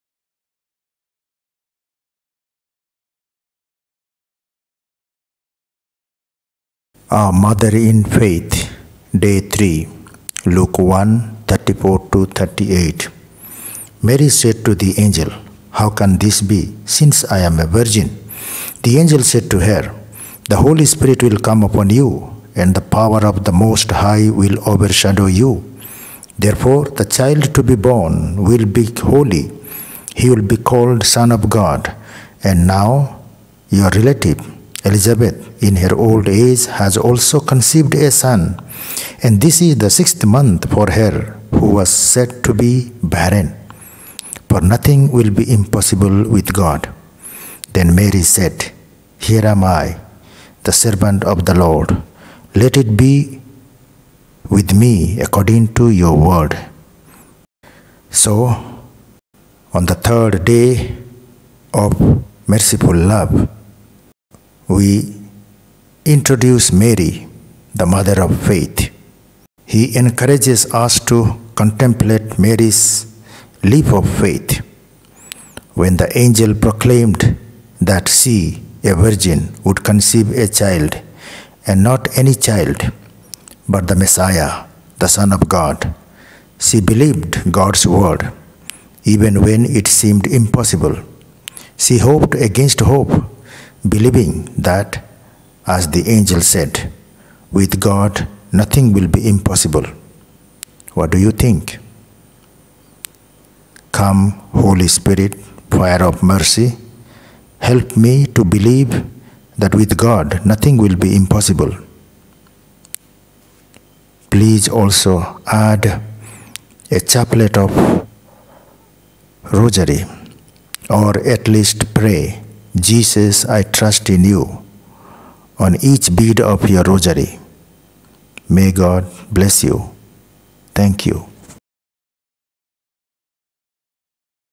DAY 3 (Thur): Our Mother in Faith | 33 Days to Divine Mercy | Week 1: What is Trust? |Fr. Lameswar.
WEEK 1 : WHAT IS TRUST ?
DAY 3 : 24 Mar (Thur)
Our Mother in Faith

For St. Therese, it’s all about trust. But what is trust? That’s what we’re going to ponder this week, using Sacred Scripture and its giants of trust. (By the way, I'm going to be using the words “trust” and “faith” pretty much interchangeably because in Therese’s teaching, they’re more or less the same.) Next week, we’re going to start learning about St. Therese and her spiritual doctrine, which builds on this week’s scriptural foundation.

Yesterday, we learned that Abraham is our father in faith — but there’s a “greater than Abraham” here. Of course, I’m talking about Mary, our mother in faith. After all, she’s the greatest example of faith, its “perfect embodiment,” and its “purest realization.” And while Jesus, as true God, is the object of our faith (the one we believe in), Mary is the supreme model of faith (the one who best shows us what it means to believe in God).

So, what about Mary’s faith? What does she show us about believing in God? Well, let me begin to answer that by first telling you an amazing secret: According to St. John Paul II, there’s “a kind of ‘key’ which unlocks for us the innermost reality of Mary.” Imagine that. According to the greatest pope of the 20th century, there’s a secret that reveals the mystery of Mary! Alright, so do you want to know what it is? Let me explain.

The great key, the great secret, has to do with Mary’s faith, because faith gets to her deepest reality'. And while we find this key in words spoken about Mary at the time of the Visitation, the words themselves refer back to her response to the angel at the Annunciation. Let’s begin by reviewing that scene:

In the sixth month the angel Gabriel was sent from God to a city of Galilee named Nazareth, to a virgin betrothed to a man whose name was Joseph, of the house of David; and the virgin’s name was Mary. And he came to her and said, “Hail, full of grace, the Lord is with you!” But she was greatly troubled at the saying, and considered in her mind what sort of greeting this might be. And the angel said to her, “Do not be afraid, Mary, for you have found favor with God. And behold, you will conceive in your womb and bear a son, and you shall call his name Jesus. He will be great, and will be called the Son of the Most High; and the Lord God will give him the throne of his father David, and he will reign over the house of Jacob for ever; and of his kingdom there will be no end.” And Mary said to the angel, “How can this be, since I have no husband?” And the angel said to her, “The Holy Spirit will come upon you, and the power of the Most High will overshadow you; therefore the child to be born will be called holy, the Son of God. And behold, your kinswoman Elizabeth in her old age has also conceived a son; and this is the sixth month with her who was called barren. For with God nothing will be impossible.” And Mary said, “Behold, I am the handmaid of the Lord; let it be to me according to your word.” And the angel departed from her (Lk 1:26-38).

Of course, we know the rest of the story. After the angel departs, Mary goes in haste to visit her cousin, St. Elizabeth (the Visitation), and when Elizabeth hears Mary’s greeting, the child in her womb leaps for joy. But then, Elizabeth gives the whole world a key to Mary’s innermost reality. “Filled with the Holy Spirit” and “with a loud cry,” she proclaims two blessings, the last of which is the great key. Here it is: “Blessed is she who believed that what was spoken to her by the Lord would be fulfilled” (Lk 1:45).

Now, according to St. John Paul II, that final blessing has a “fundamental importance.” Why? Because its words reveal the secret of Mary’s faith, which is this: She BELIEVED that what the Lord had spoken to her at the Annunciation would be fulfilled. And so, her faith is like that of Abraham, as John Paul points out:
Just as Abraham “in hope believed against hope, that he should become the father of many nations” (Rom 4:18), so Mary, at the Annunciation, having professed her virginity (“How shall this be, since I have no husband?”) believed that through the power of the Most High, by the power of the Holy Spirit, she would become the Mother of God’s Son in accordance with the angel’s revelation: “The child to be born will be called holy, the Son ofGod” (Lk 1:35).

So, if we want to understand the mystery of Mary, we  should reflect on the fact that she believed God’s Word, even when it seemed impossible. She hoped against hope, believing that, as the angel said, “with God nothing will be impossible.”

PRAYER:
Come, Holy Spirit, fire of mercy.
Help me to believe that with God
nothing will be.

EDITING APP:
Vita

IMAGE USED-
Picryl
Wikipedia